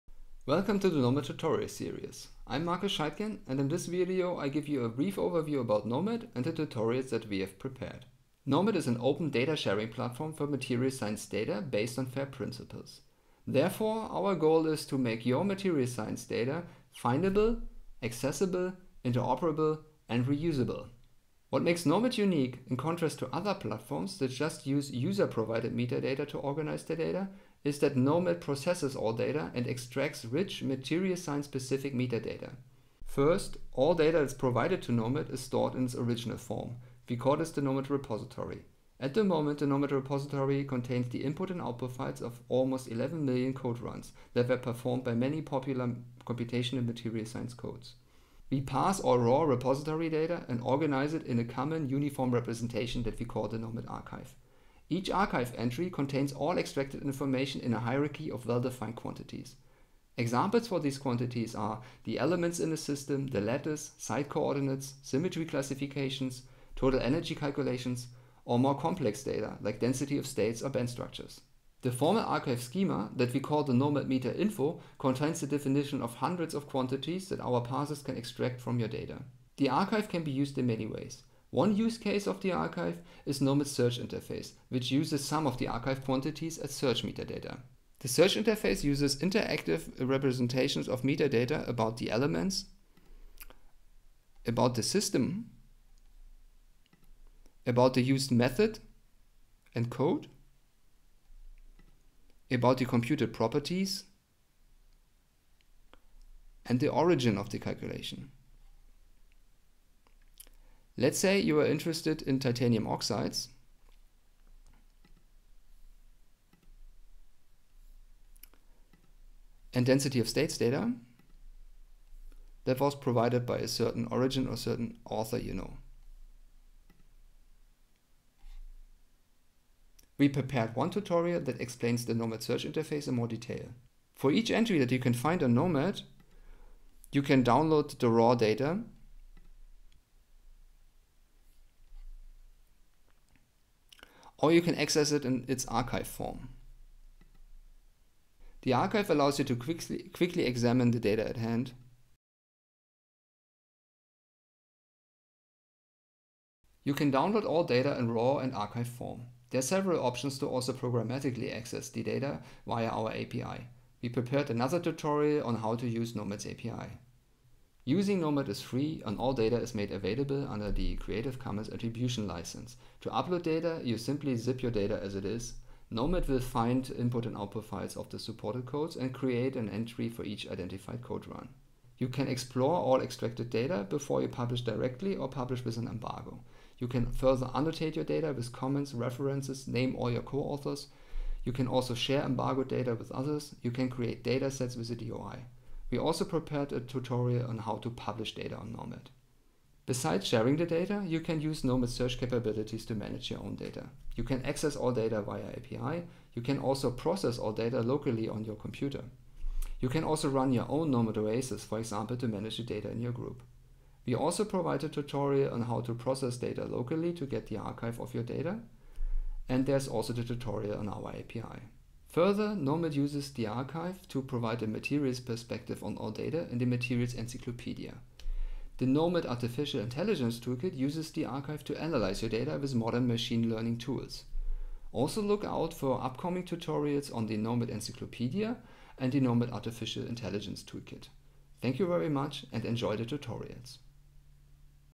NOMAD introduction (virtual tutorial series)
This is part of the 1st tutorial in the NOMAD virtual tutorial series. This is on the NOMAD Repository and Archive. You find the web-site for the tutorial

This is the introduction video, where NOMAD Developer Markus Scheidgen explains what NOMAD is and what role NOMAD components like the Repository and Archive play. Before you start to watch the other tutorial videos and do the exercises, we recommend to watch this introduction. It will give you a brief overview of NOMAD, its various parts and functionality as well as some nomenclature.

You find the web-based user interface to the official NOMAD Repository and Archive